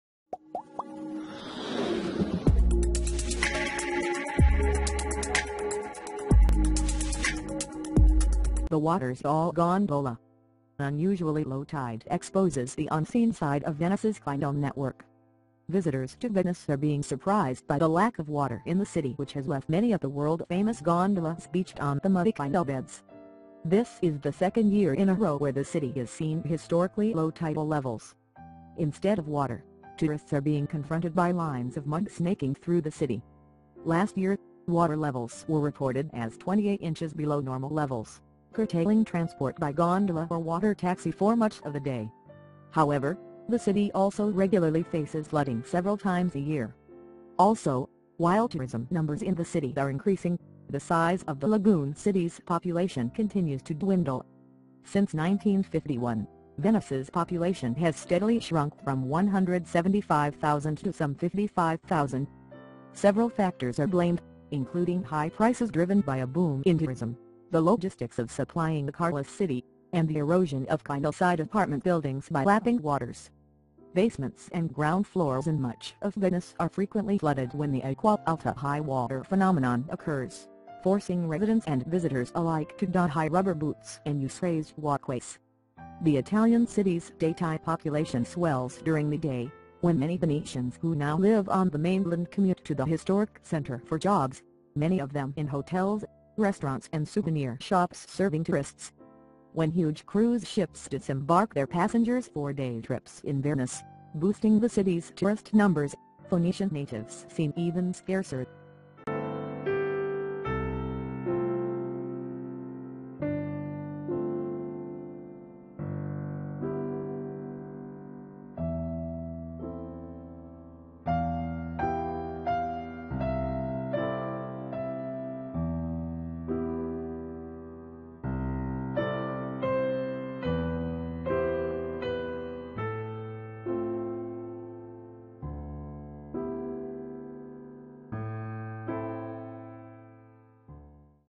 The water's all gone-dola! Unusually low tide exposes the unseen side of Venice's.
The water's all gone-dola! Unusually low tide exposes the unseen side of Venice's canal network.
Visitors to Venice are being surprised by the lack of water in the city which has left many of the world-famous gondolas beached on the muddy canal beds.
This is the second year in a row where the city has seen historically low tidal levels.
Instead of water, tourists are being confronted by lines of mud snaking through the city.
Last year, water levels were reported as 28 inches below normal levels, curtailing transport by gondola or water taxi for much of the day.
However, the city also regularly faces flooding several times a year.
Also, while tourism numbers in the city are increasing, the size of the lagoon city's population continues to dwindle.
Since 1951, Venice's population has steadily shrunk from 175,000 to some 55,000.
Several factors are blamed, including high prices driven by a boom in tourism, the logistics of supplying a carless city, and the erosion of canal-side apartment buildings by lapping waters.
Basements and ground floors in much of Venice are frequently flooded when the 'acqua alta' (high water) phenomenon occurs, forcing residents and visitors alike to don high rubber boots and use raised walkways.
The Italian city's daytime population swells during the day, when many Venetians who now live on the mainland commute to the historic center for jobs, many of them in hotels, restaurants and souvenir shops serving tourists.
When huge cruise ships disembark their passengers for day trips in Venice, boosting the city's tourist numbers, Venetian natives seem even scarcer.